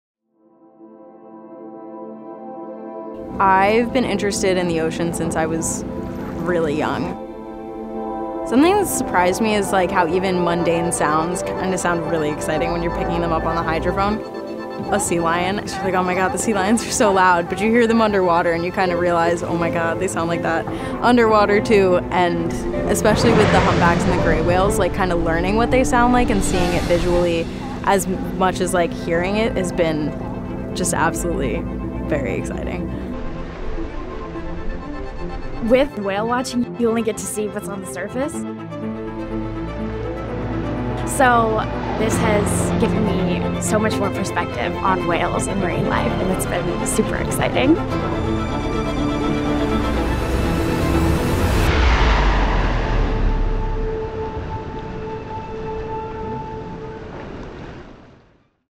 Cal Poly Students Surprised by Humpback Whales While Collecting Data
4th-year marine science majors Adelle Wilkin and Sophie Short describe how they’re using audio data analysis to learn more about our undersea neighbors.